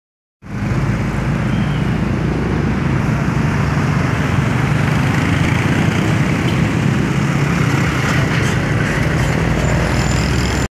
Jalan Alternatif Sentul Lancar, Senin (8/8/2016).
Arus kendaraan didominasi truk pengangkut barang